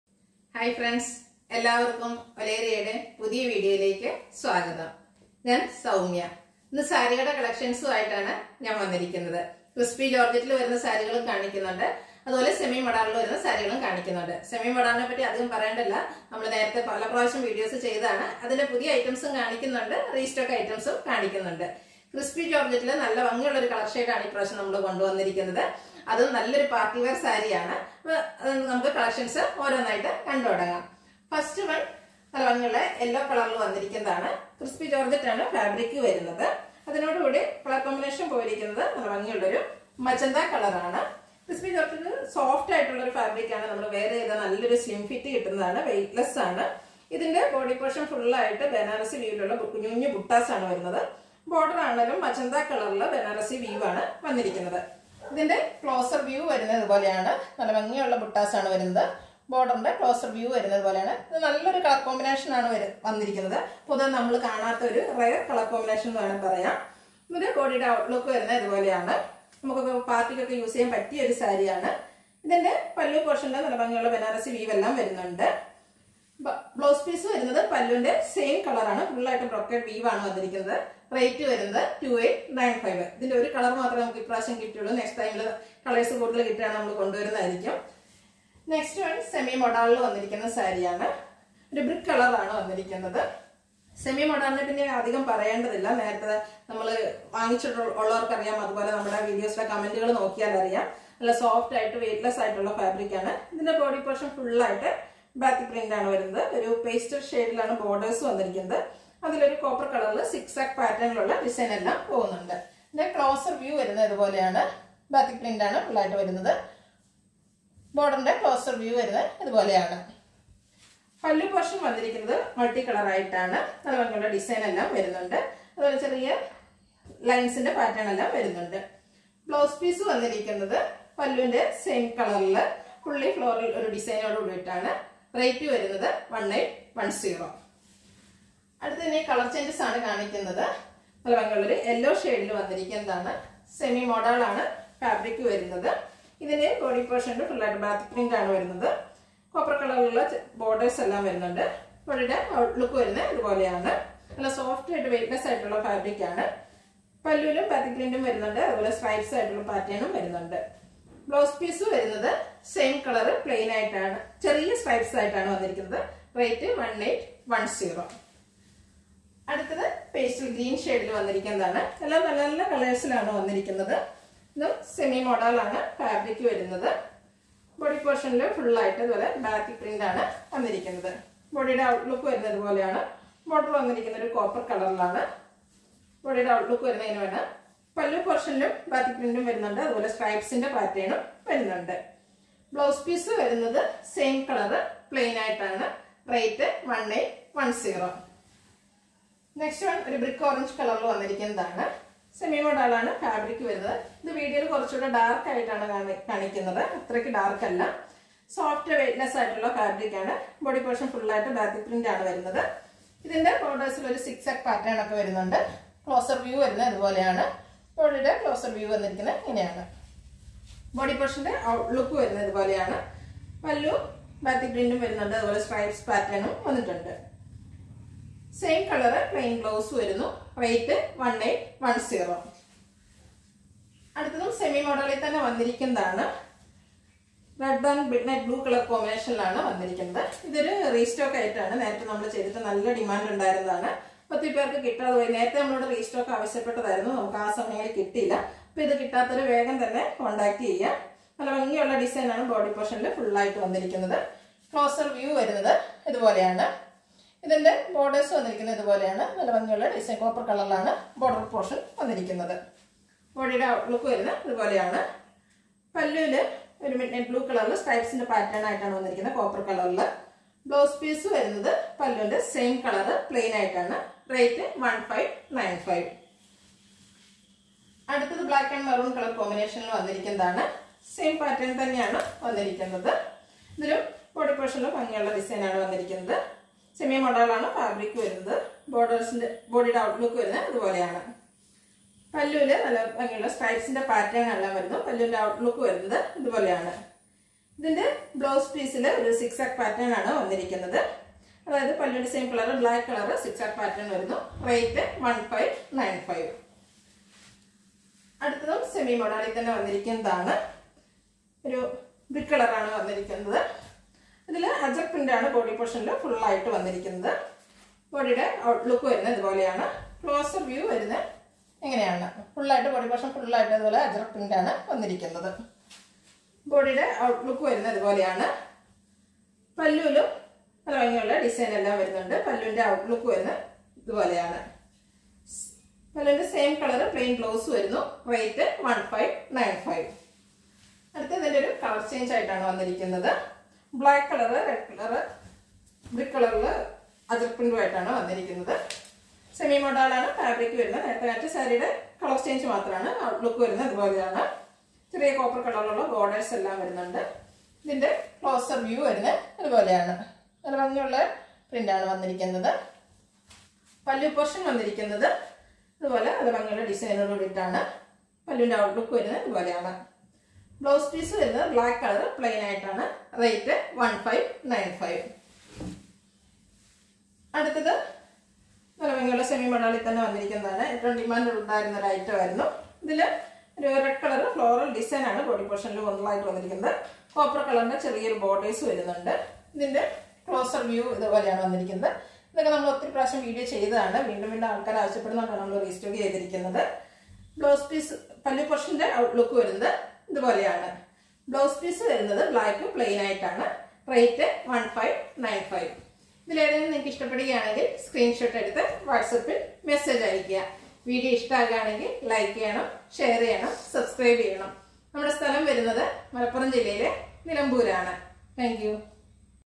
നിങ്ങൾ വീണ്ടും ആവശ്യപ്പെട്ട സാരി കളക്ഷൻ//Booking : 8848089032, 9074248160
RESTOCKED
Semi Modal Silk Sarees & Crispy Georgette Sarees
Rate :Starting @ 1595/-
Blouse Piece Attached

Payment : Google Pay, Phone Pay, Bank Transfer

Location : Edivanna
                   Nilambur
                   Malappuram
                   Kerala
Pincode  : 679329